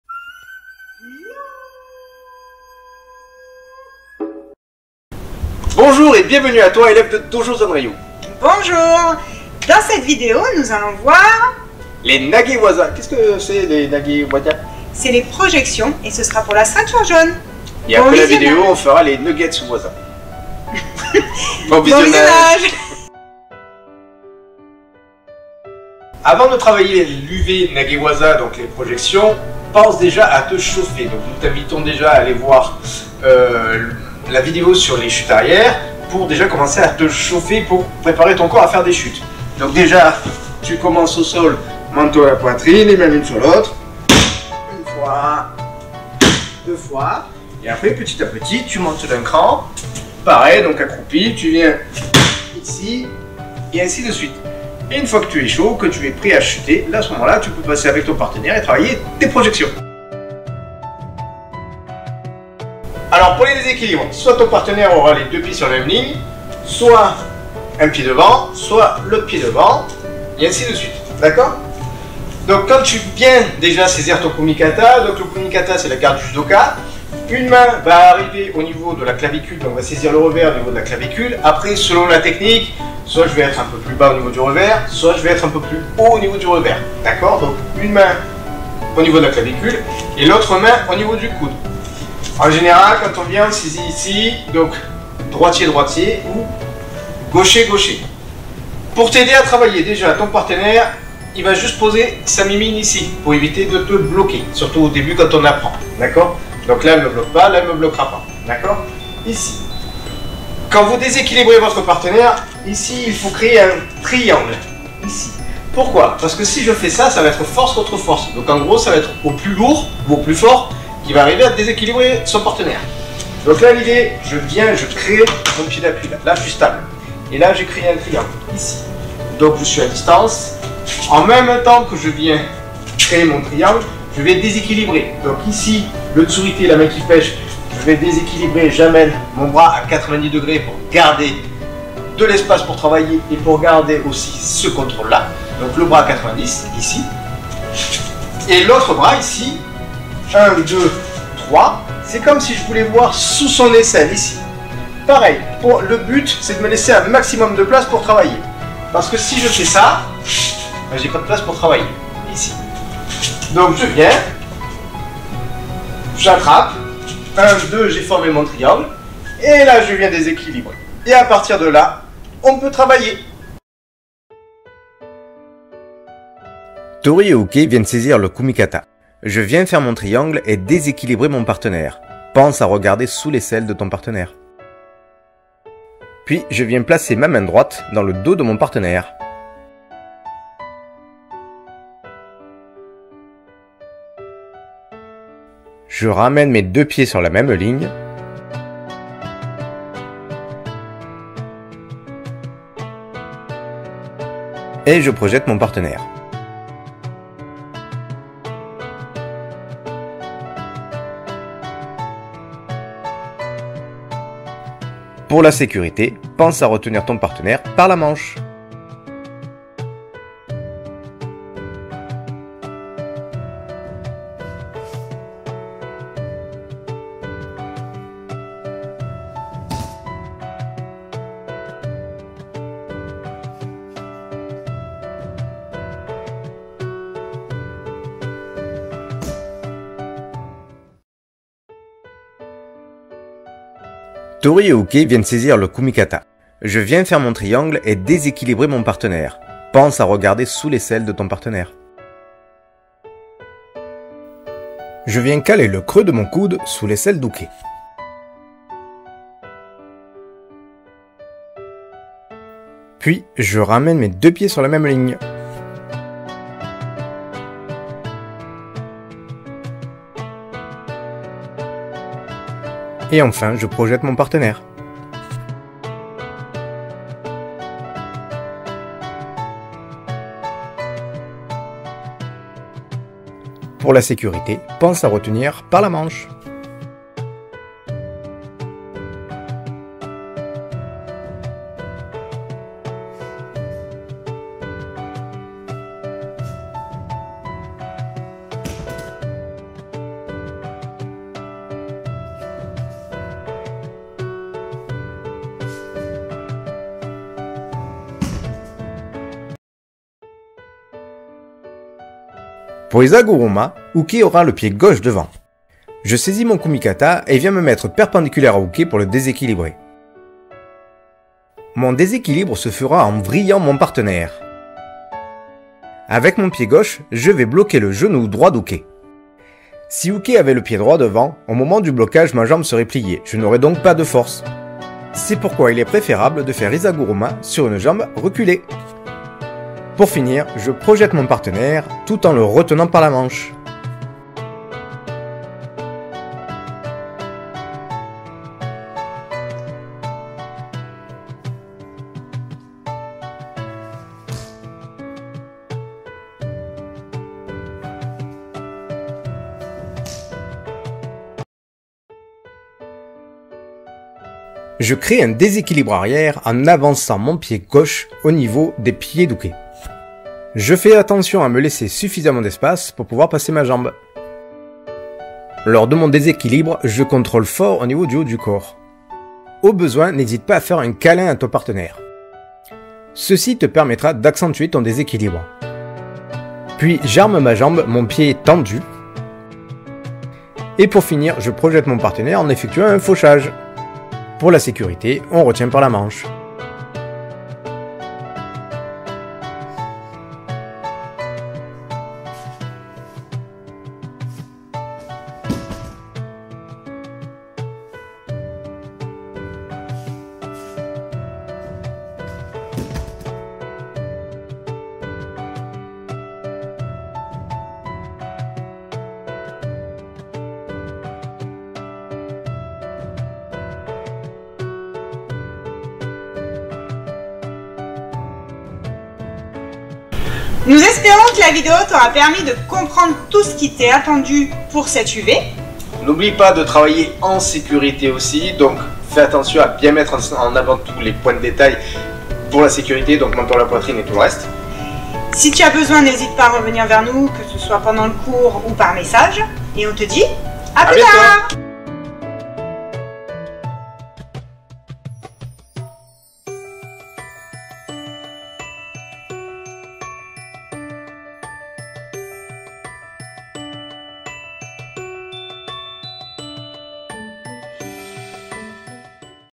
Dojo Zen Ryu - UV6 Les NAGE WAZA à présenter pour la ceinture jaune
Nous avons fait cette vidéo pour nos élèves du Club Dojo Zen Ryu souhaitant passer leur UV6 (les NAGE WAZA (Techniques de projections)), pour la ceinture jaune.

Toutefois ce support pédagogique est ouvert à tout le monde !

Nous espérons qu'il vous aidera dans la préparation de votre UV.
Nous sommes à votre disposition pour toutes questions.

Nous remercions le club de l'EAMC, pour la mise à disposition du Dojo.

Véro et Marc Dojo Zen Ryu

Sommaire :
00:00 Intro
00:23 Avant de commencer
01:00 Point de départ
02:53 O GOSHI
03:39 Mise en application
04:01 IPPON SEOI NAGE
04:48 Mise en application
05:09 HIZA GURUMA
05:45 Mise en application
06:01 O SOTO GARI
06:37 Mise en application
06:54 ATTENTION
07:07 Remerciements
06:29 Attention